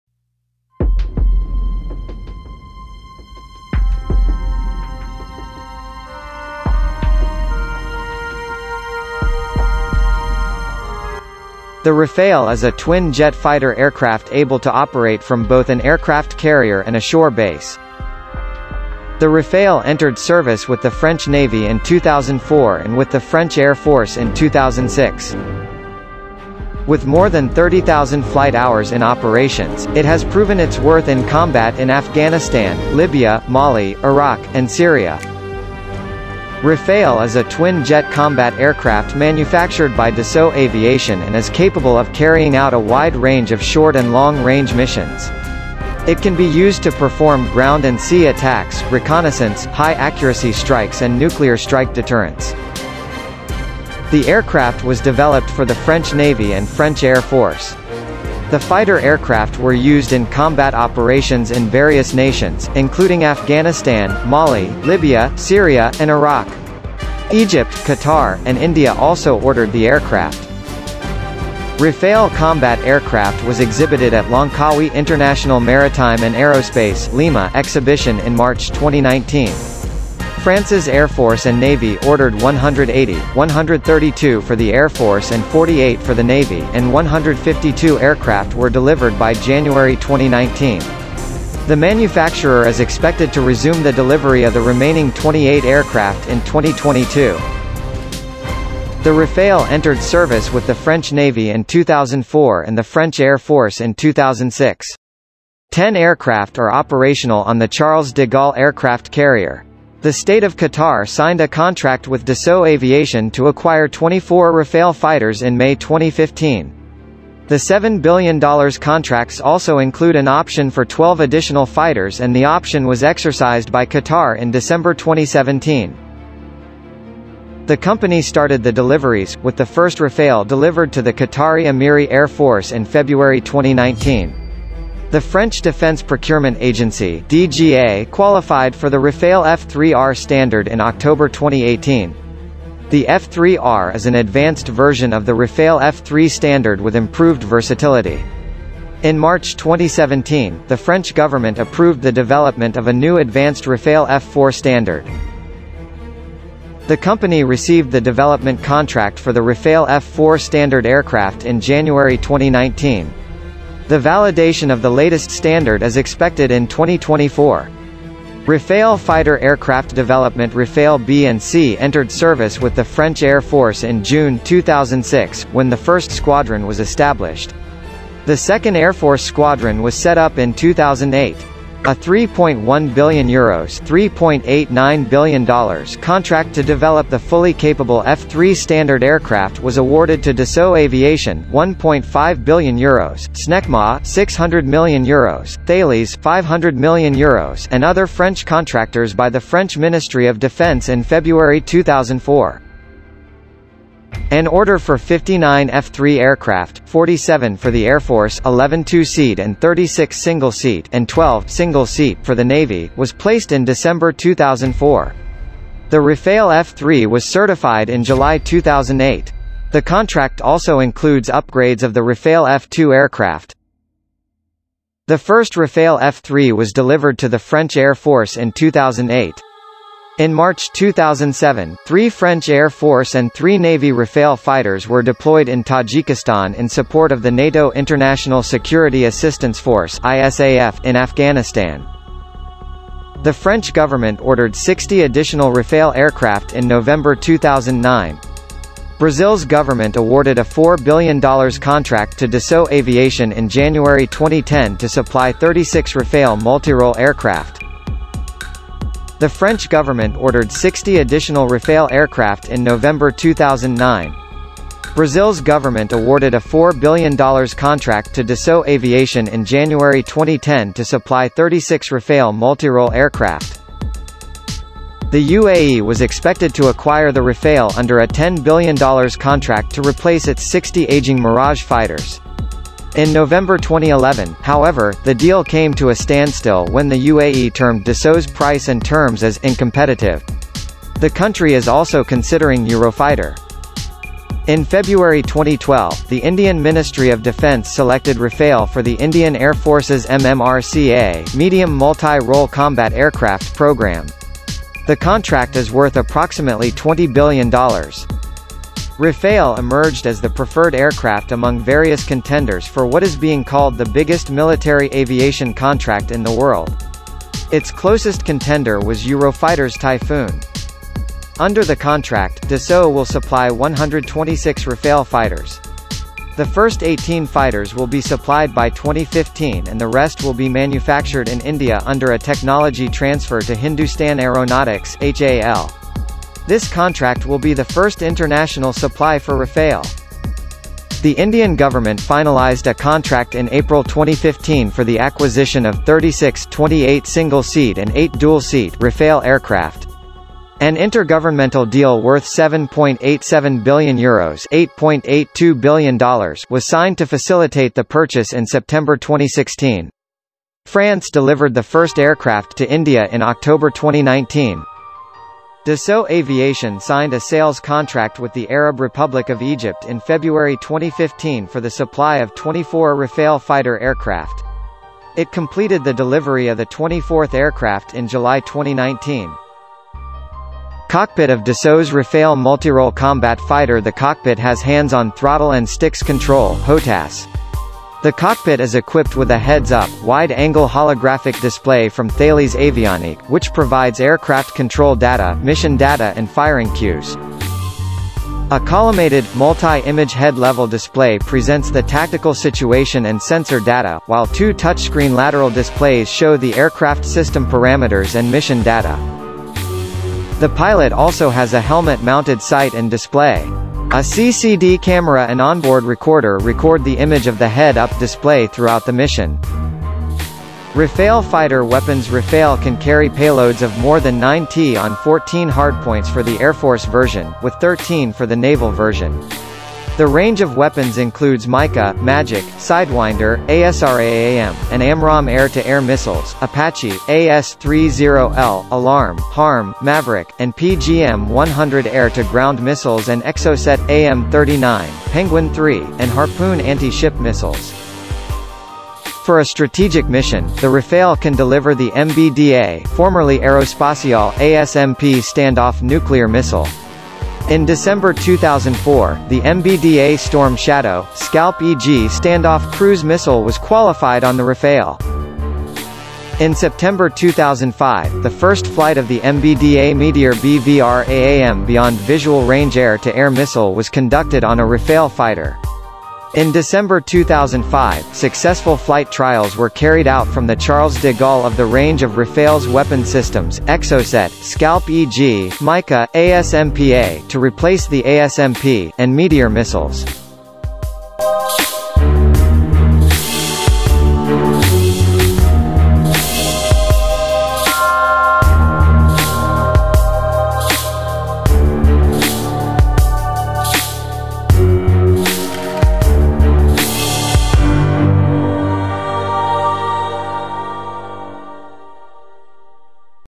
rafale was developed for the French Navy and French Air Force
The aircraft was developed for the French Navy and French Air Force

The Rafale is a twin-jet fighter aircraft able to operate from both an aircraft carrier and a shore base.

The Rafale has been cleared to operate the following weapons: The MICA air-to-air “Beyond Visual Range” (BVR) interception, combat and self-defense missiles, in their IR (heat-seeking) and EM (active radar homing) The MICA can be used within visual range (WVR) and beyond visual range (BVR).
The notion of aircraft generations, a term that applies to the only jet rather than propeller-driven fighter aircraft, appeared in the 1990s and attempted to make sense of the leap-frogging improvements in performance to jet fighter aircraft brought about through major advances in aircraft design, avionics, and weapon systems.

While the rationale that constitutes a generational shift is debatable, a generational shift in jet fighter aircraft occurs when a technological innovation cannot be incorporated into an existing aircraft through upgrades and retrospective fit-outs.